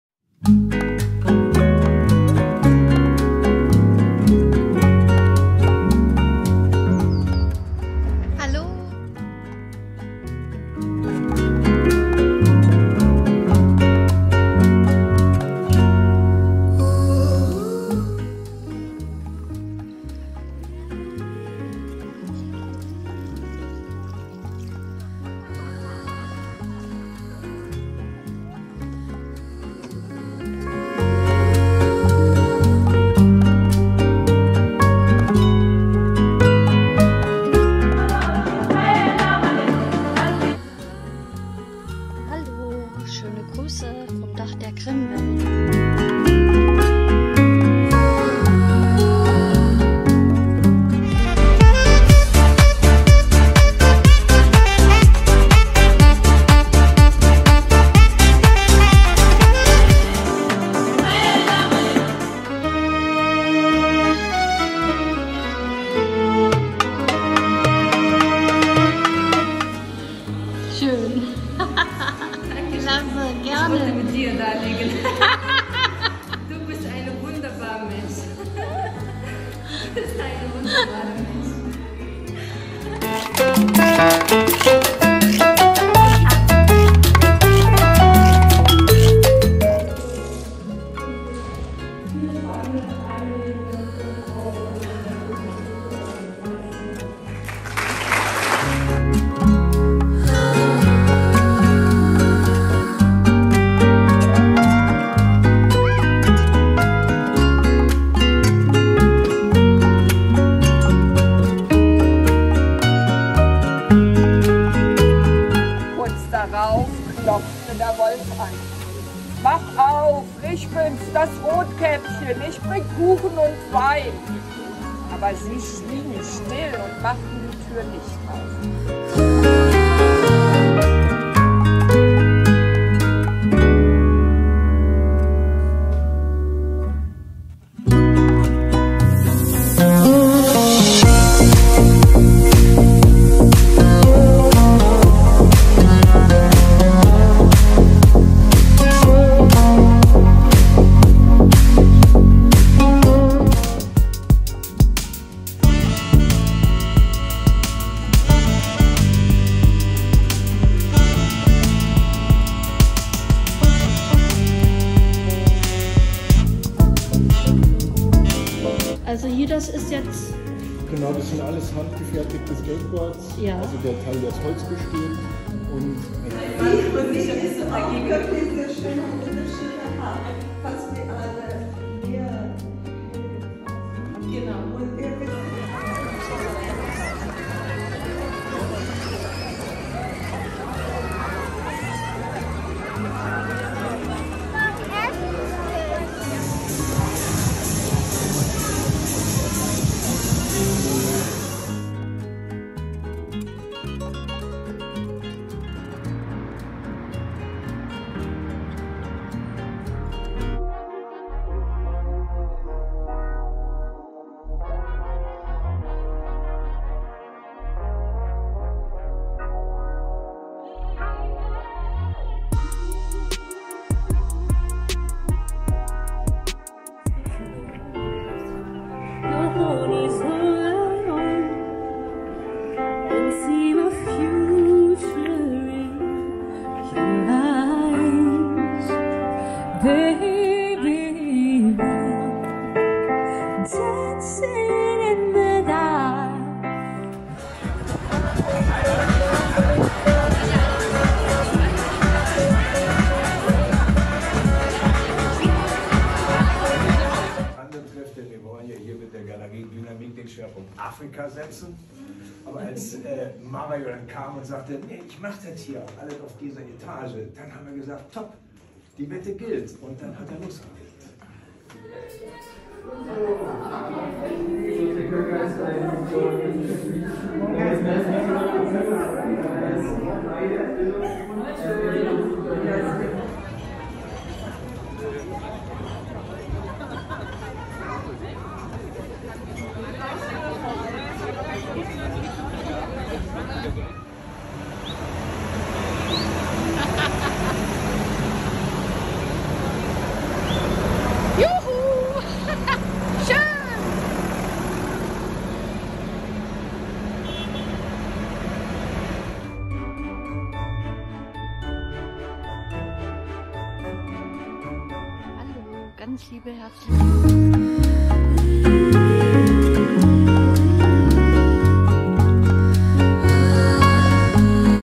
Jahresrückblick 2021 -KKK- Kunst/Kultur/Kassel
Mix Veranstaltungen und Begegnungen in 2021, ob Beuys 100, Vernissage mit Artvanced. Vernissage mit Violett Kunstgalerie, Documantahalle oder im Botanischen Garten, Erzählkunst  und mehr. Im Rückblick waren es für mich viele  interessante Eindrücke.
Freue mich wenns Euch gefällt. LG Doris